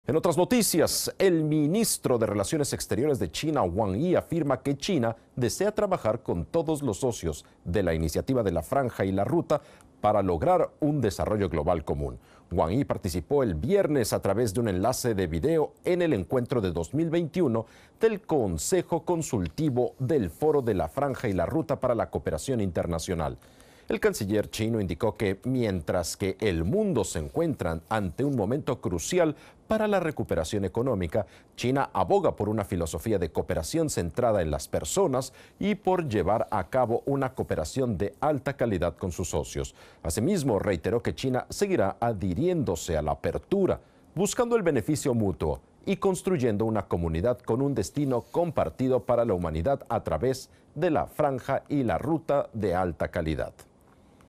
Wang Yi subraya la importancia de la cooperación de alta calidad para el desarrollo común
El ministro de Relaciones Exteriores de China, Wang Yi, afirma que China desea trabajar con todos los socios de la Iniciativa de la Franja y la Ruta para lograr un desarrollo global común. Wang Yi participó el viernes 17 de diciembre a través de un enlace de vídeo en el encuentro de 2021 del Consejo Consultivo del Foro de la Franja y la Ruta para la Cooperación Internacional.